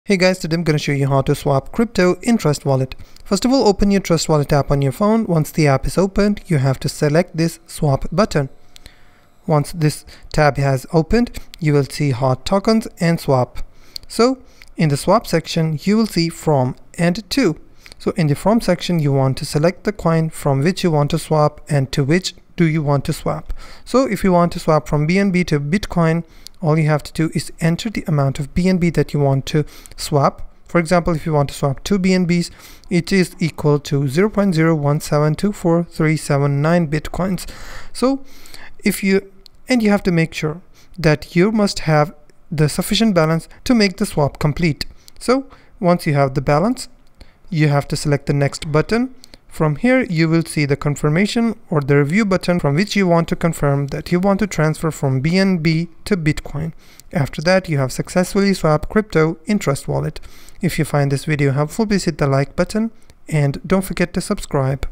🔄 How To Swap Crypto In Trust Wallet | Step-by-Step (2025)
👉 In this tutorial, we will show you how to easily swap cryptocurrencies in Trust Wallet 🔄 Learn how to exchange coins seamlessly using Trust Wallet's intuitive interface and take control of your crypto portfolio! cryptocurrency trustwallet swapcoins 💰📱